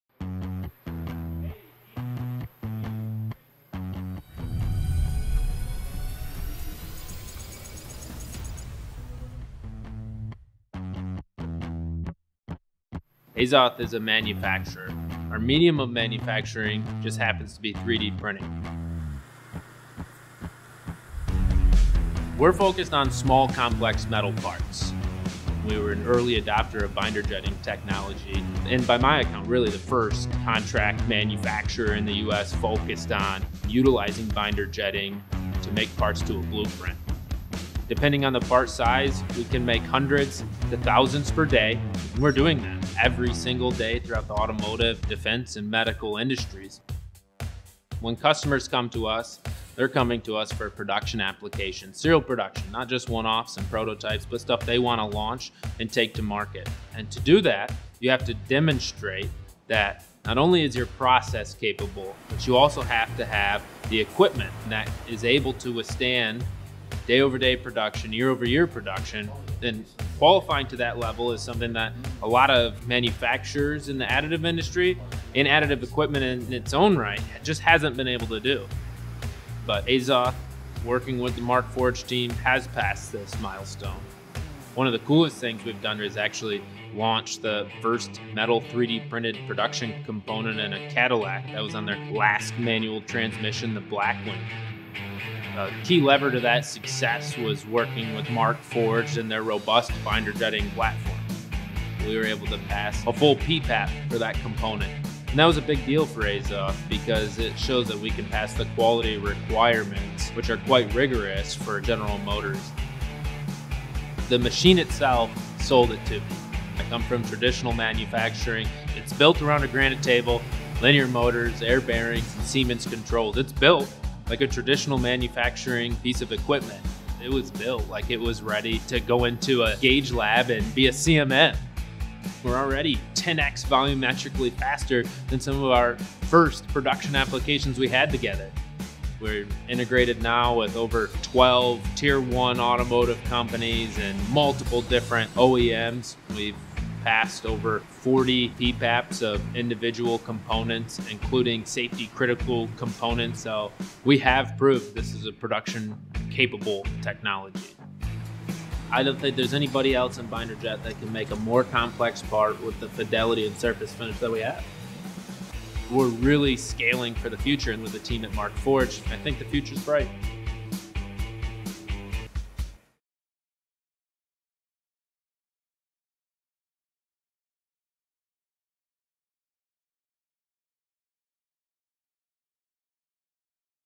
3D Printing Car Parts for General Motors with Azoth 3D | Markforged in the Field
Azoth 3D is a trusted, Michigan-based manufacturer known for producing small, complex metal parts. And they use additive technology like Markforged binder jetting to get the job done.

The team at Azoth used their binder jetting expertise to deliver for General Motors, one of the many customers they serve across the automotive, defense, and medical industries. They designed and produced a 3D printed medallion that sits on top of the manual shifter knob in their Cadillac Blackwing vehicles.

It’s the first 3D printed production component in a Cadillac.

“A lot of manufacturers in the additive industry, and additive equipment in its own right just hasn’t been able to do [that]. But Azoth, working with the Markforged team, has passed this milestone,” says General Manager, Cody Cochran.

By utilizing the Markforged Binder Jetting solution, Azoth 3D has been able to pass a full PPAP (Production Part Approval Process) on more than 40 parts, including safety critical components.